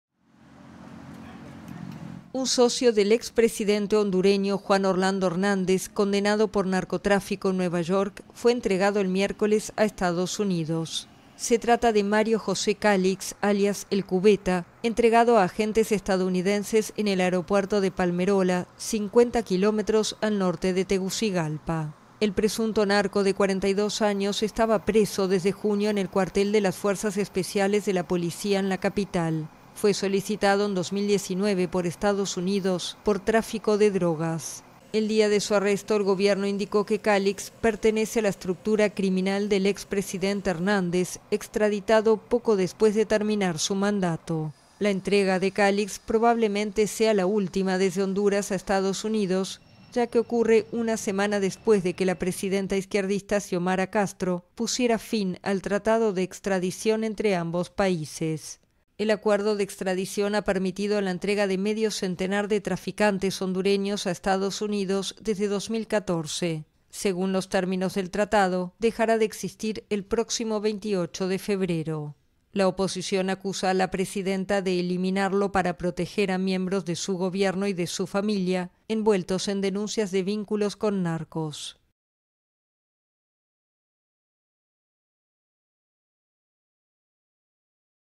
Honduras entrega a EUA a presunto narco tras denuncia del tratado de extradición
Un socio del expresidente hondureño Juan Orlando Hernández, condenado por narcotráfico en Nueva York, fue entregado este miércoles a Estados Unidos, una semana después de que la mandataria izquierdista Xiomara Castro pusiese fin al tratado de extradición entre ambos países.